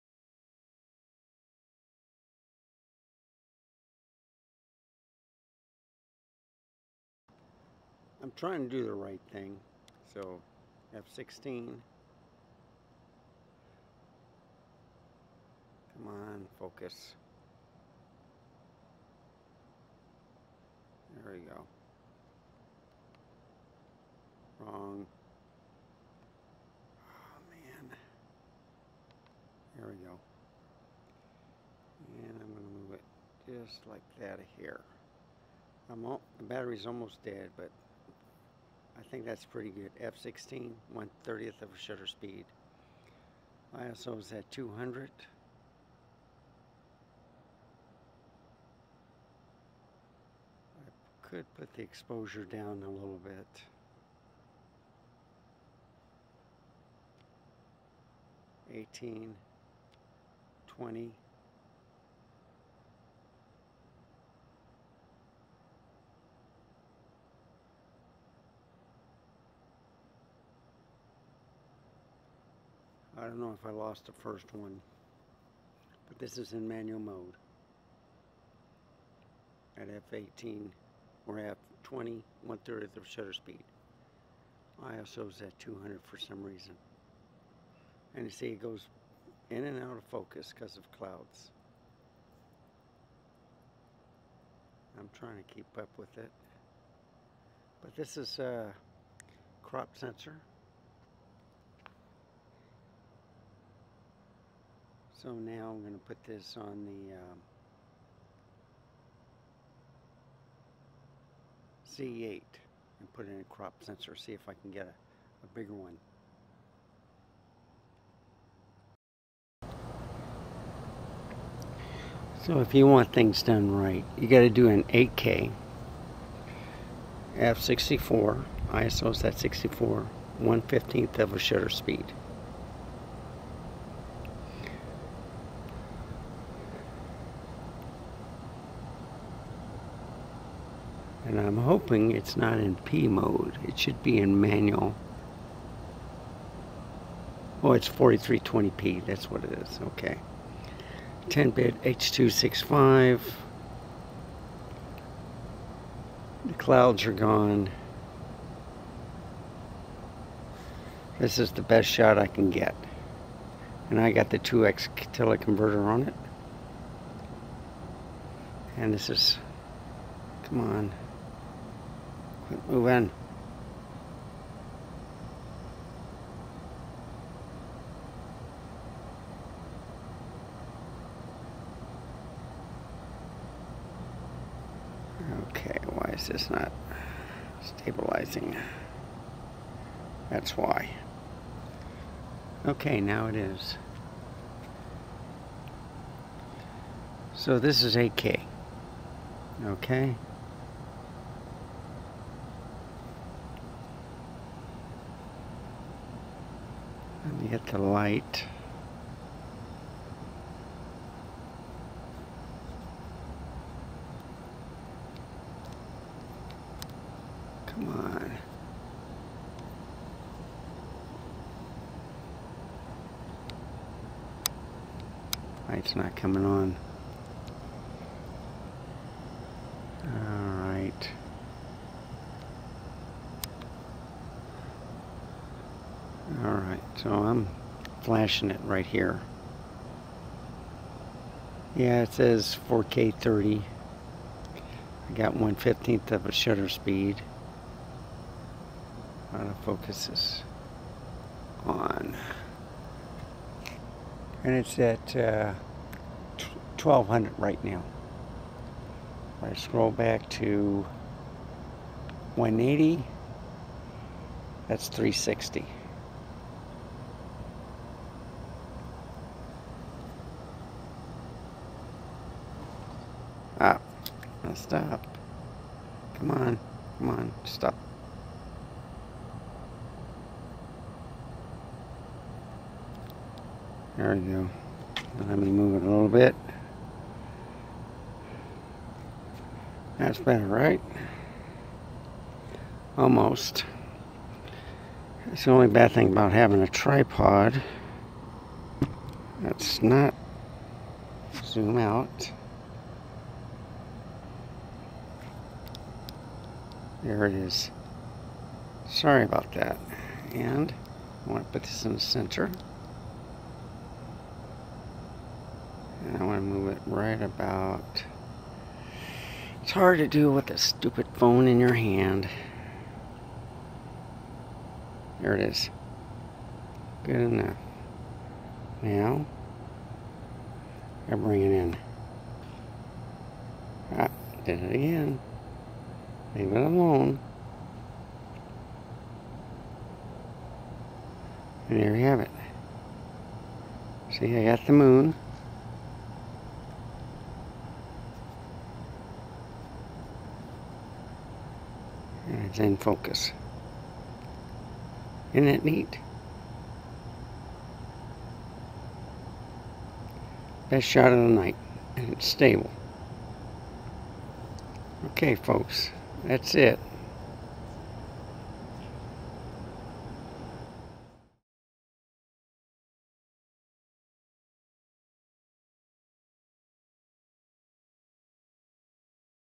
Nikon Z50 & Z8 Harvest Moon on 2024/09/20
Using the APSC Nikon Z50 Z 180-600mm Telephoto Zoom Lens at Night Time.   Then I decided to use the same lens with the Nikon Z8.  Stay Safe and Keep Smiling!  Cheers!